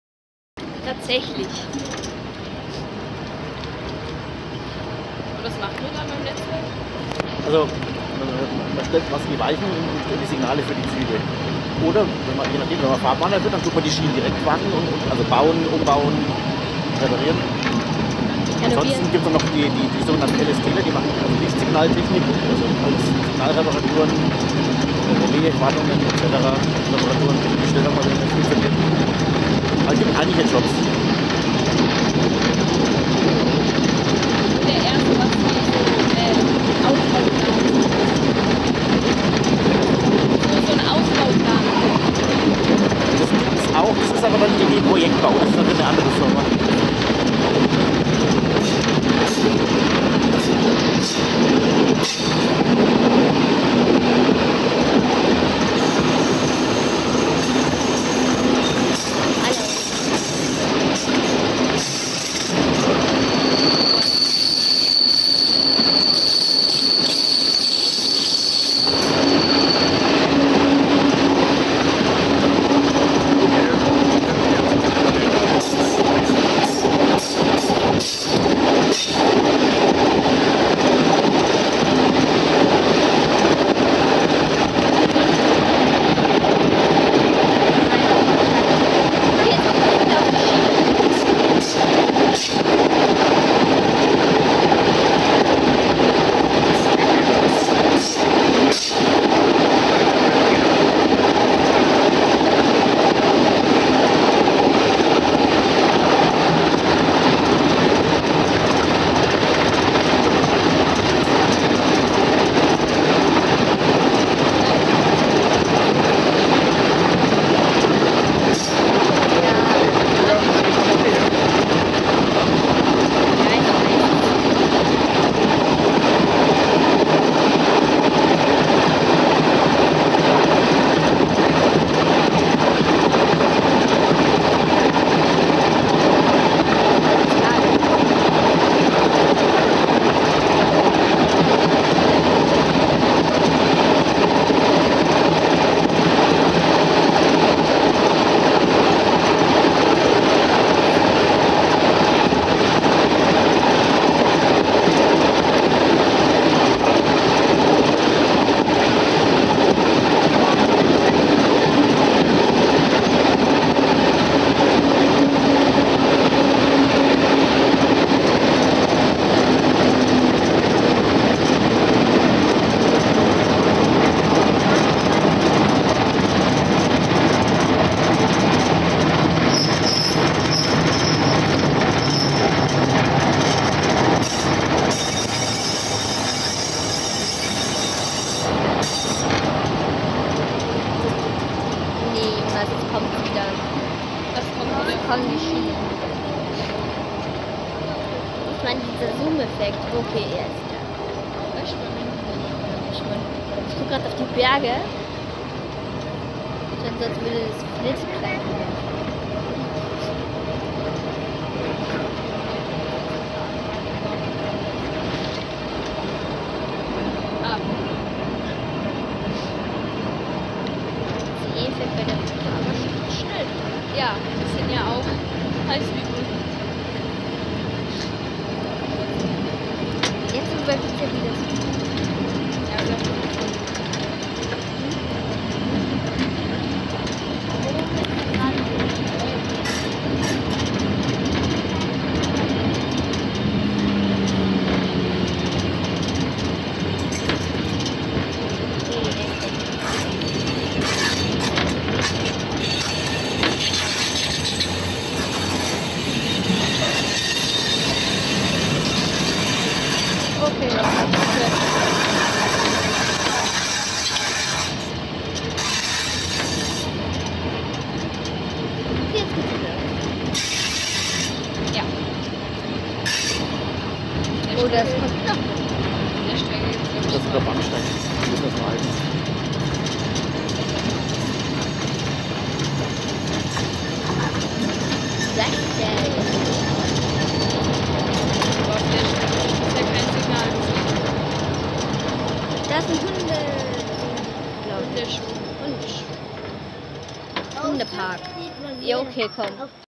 Streitberg - Gasseldorf
Mitfahrt in der DFS zum Saisonauftakt 2018